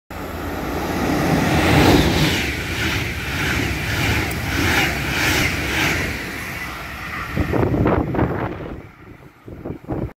Metronom von Hannover nach Göttingen am Bahnhof Rethen im Nebel
Metronom fährt von Hannover durch Sarstedt, Nordstemmen, Alfeld, Einbeck und Nothheim nach Göttingen.